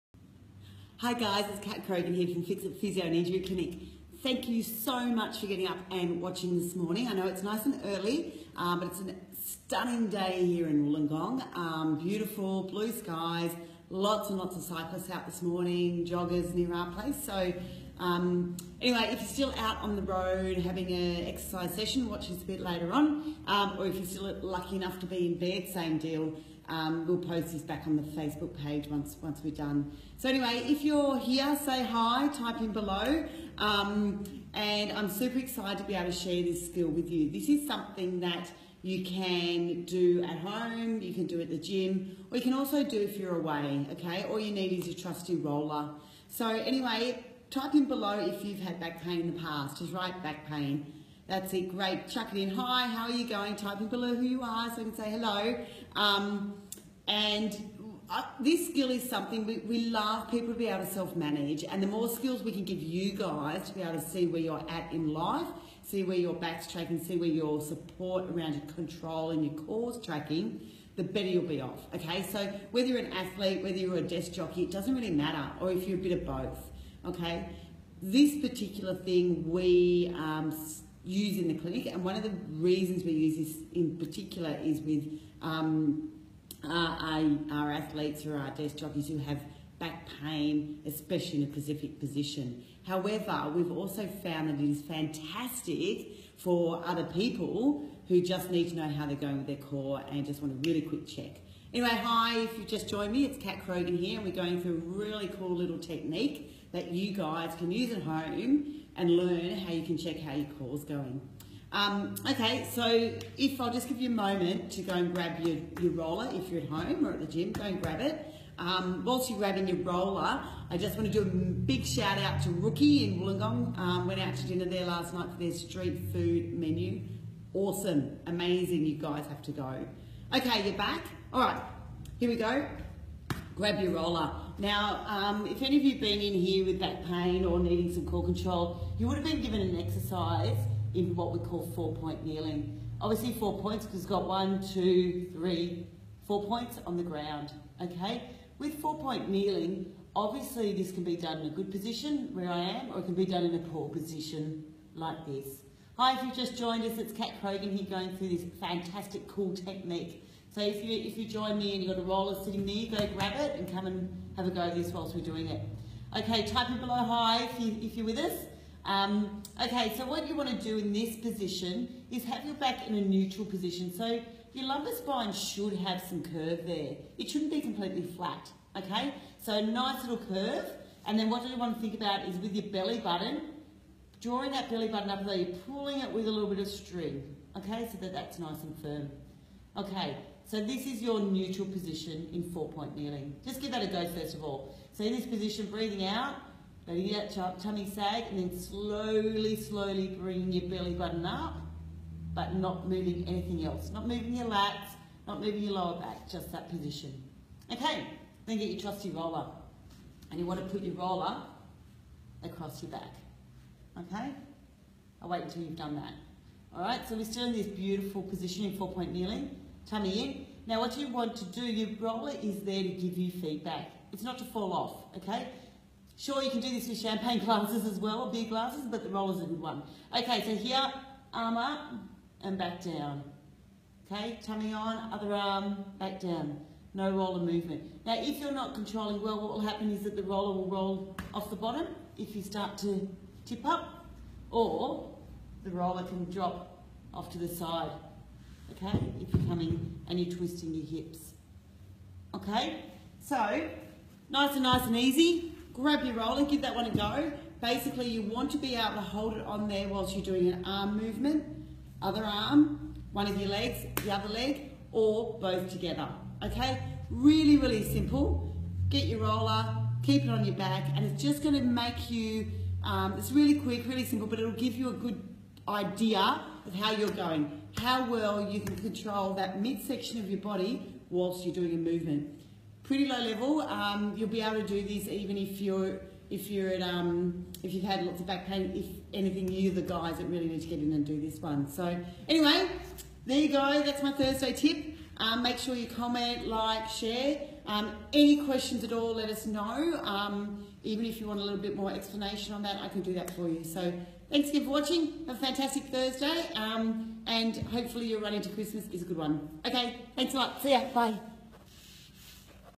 No more sleeps ! YAY
So glad you could join Cat in learning more about getting your body working the best it can for you and gaining skills that you can use yourself to check how things are going.

This video is especially for back pain - whether it is occasionally or a lot. This can help athletes or a desk jockey or a bit of both.

So either sit back and watch for the first time or grab your roller and "play along".